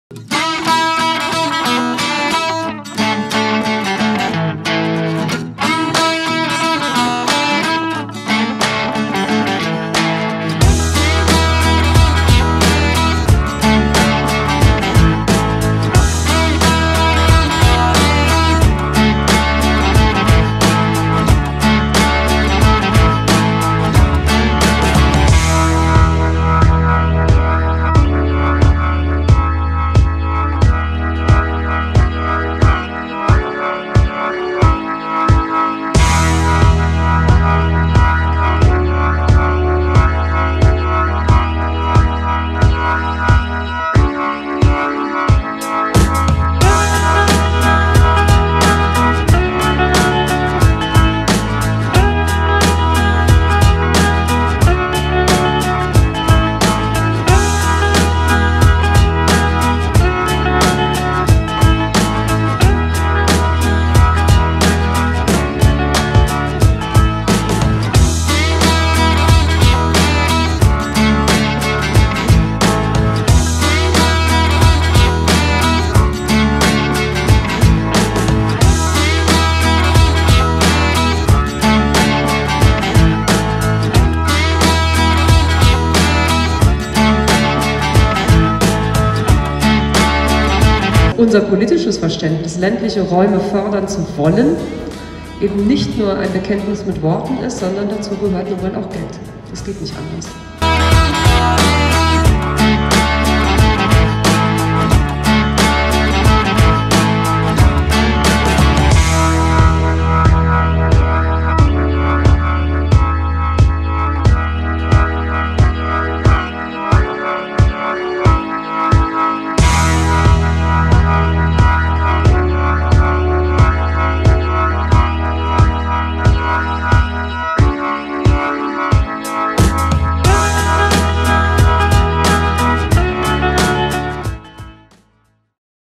Rheinische Revier-Tour: Nörvenich, Baesweiler, Kaarst, Heinsberg, Elsdorf, Titz
Jestalde, zosammehalde: Ministerin Ina Scharrenbach ist wieder unterwegs in Städten und Gemeinden, die den Wandel im Rheinischen Revier gestalten. Im Gepäck hat sie Förderungen für den Städtebau, die Heimat und Dörfer. Von Nörvenich geht es über Baesweiler, Kaarst, Heinsberg und Elsdorf bis in die Gemeinde Titz. Ihr Engagement. Unsere Leidenschaft. Heimat im Wandel.